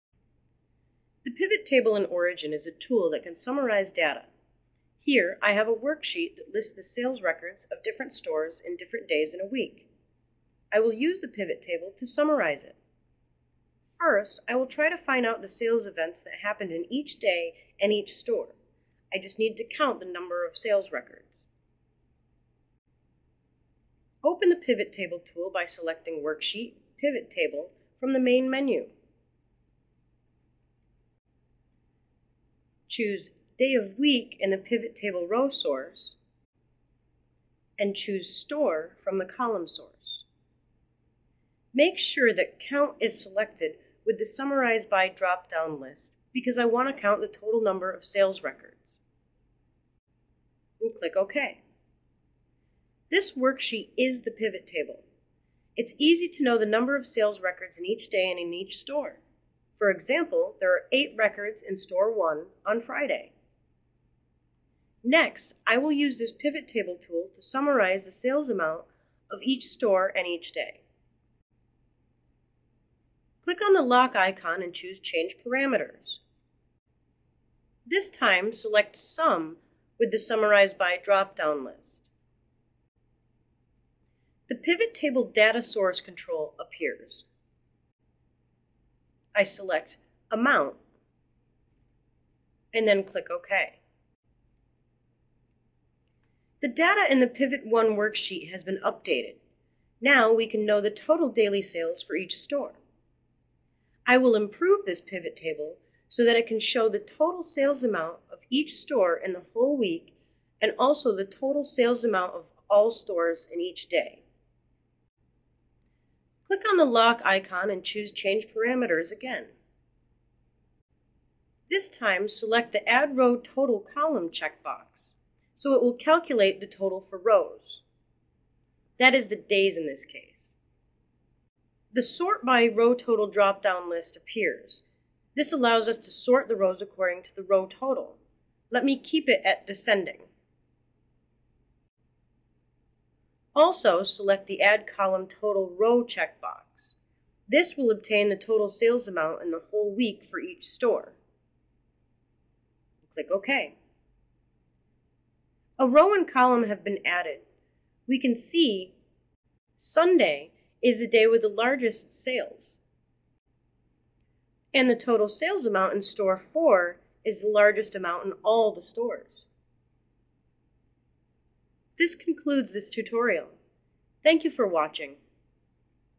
Data Manipulation: Origin: Pivot Table
Learn how to use the built-in Pivot Table tool to quickly summarize data.